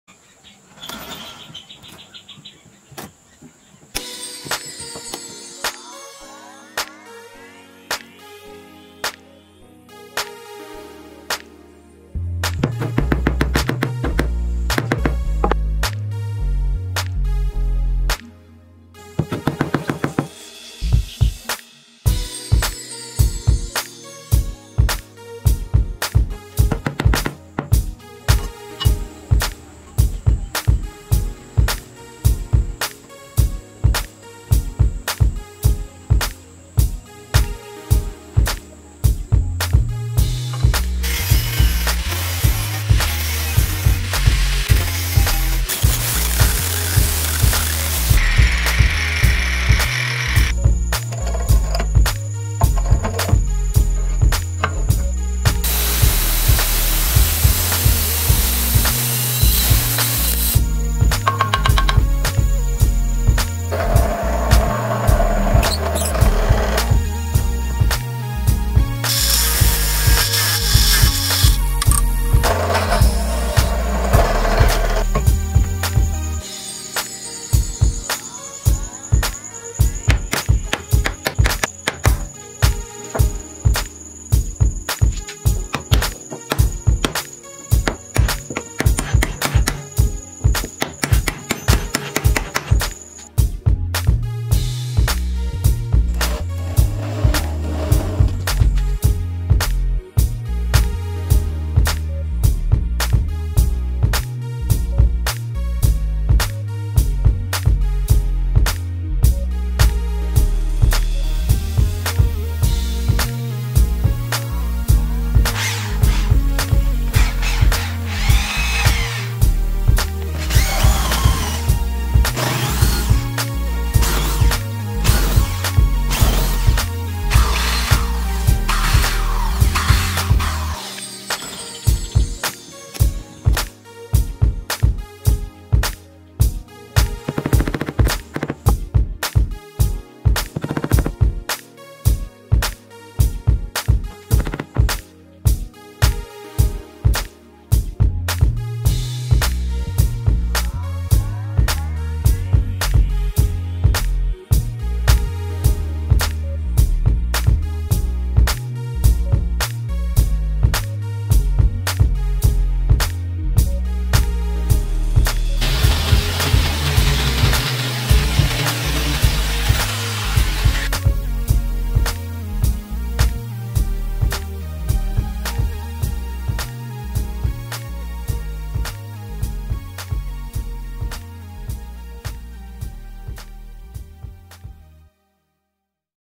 Advanced Drill Plug Cutter Tutorial: Conceal Screw Heads Beautifully with Custom Wooden Plugs!
Advanced Drill Plug Cutter Tutorial

Black Studio